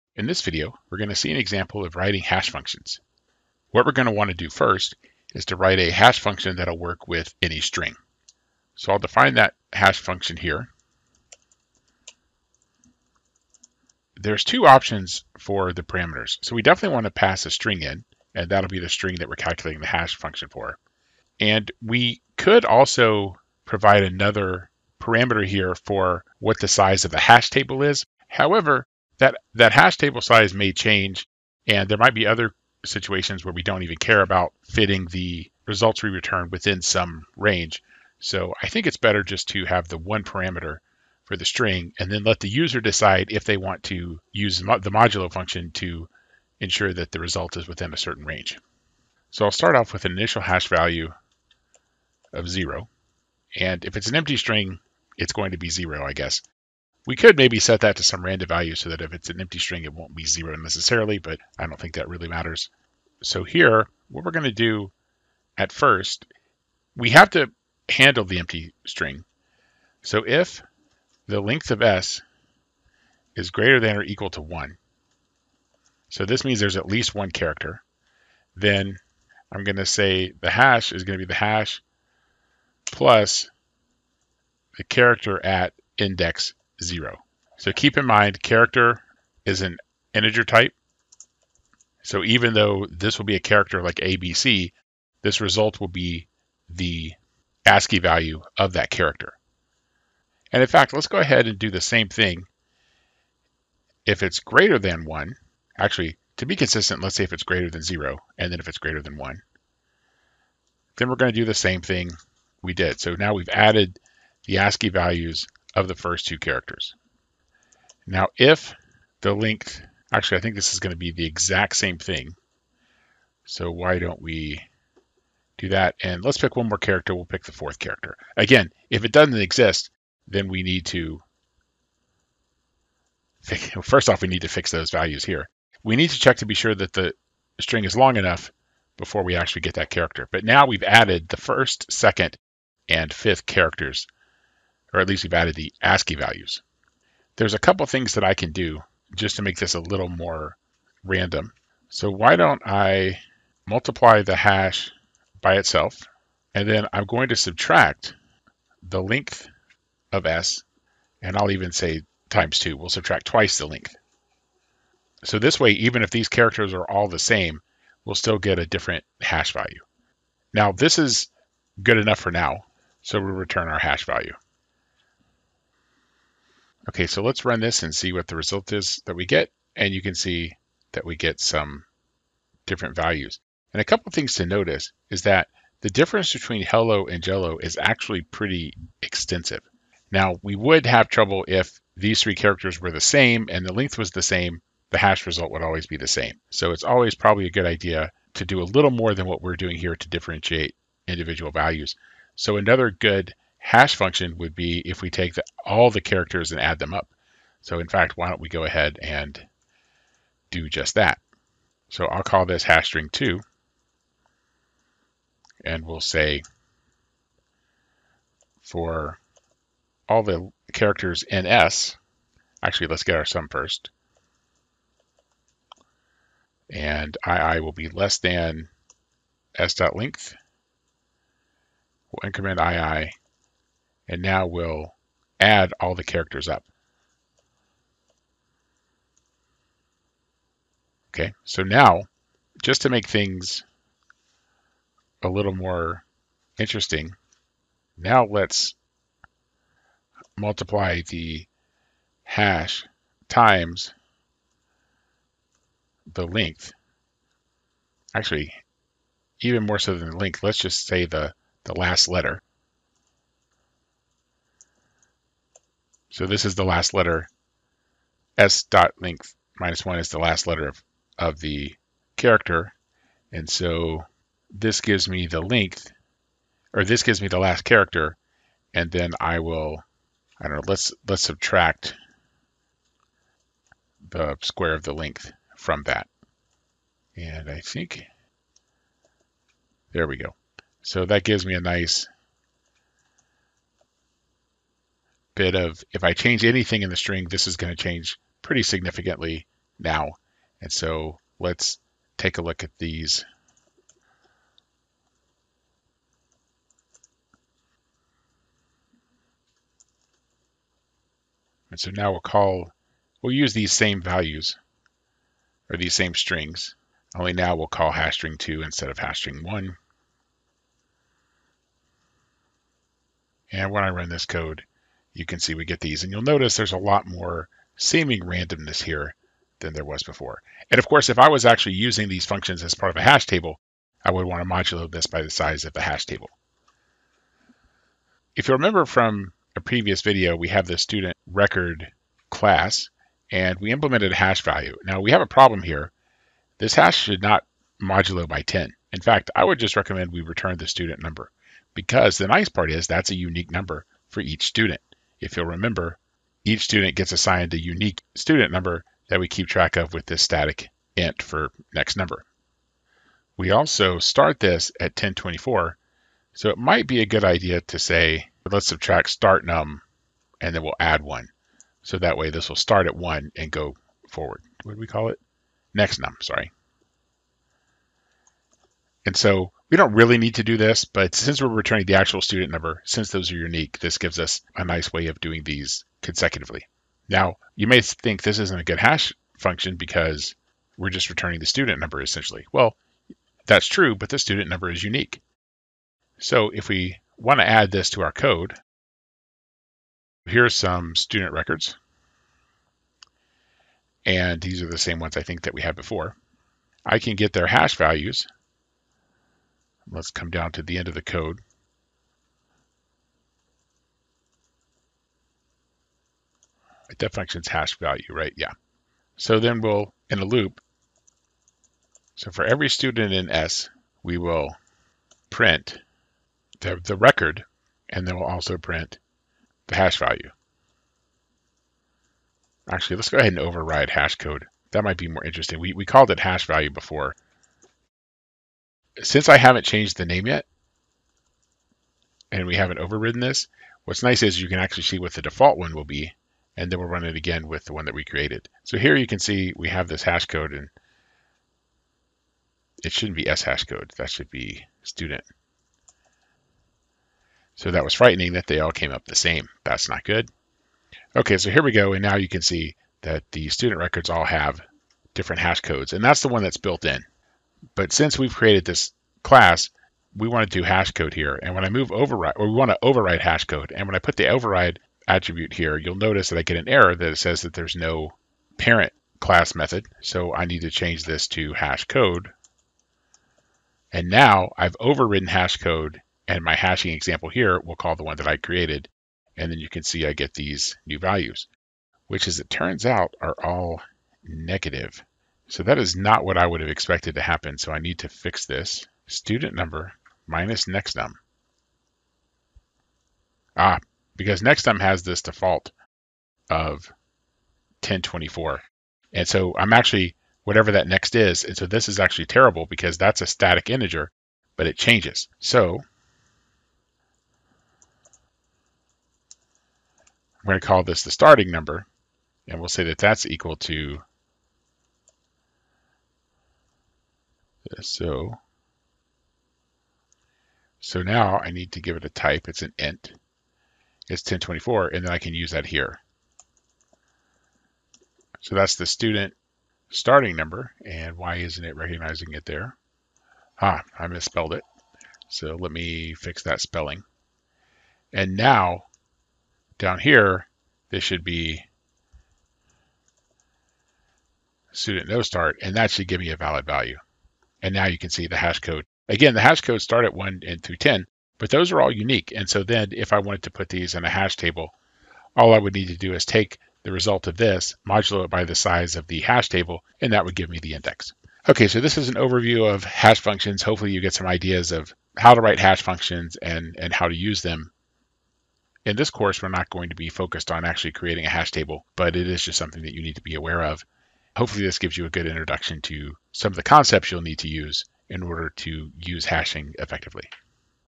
Writing Hash Functions in Java
0:10 Defining hash function for strings - hashString(s)
3:30 Defining another hash function for strings - hashString2(s)
5:38 Defining a hash function for a user-defined class (Student Record) & overloading hashCode()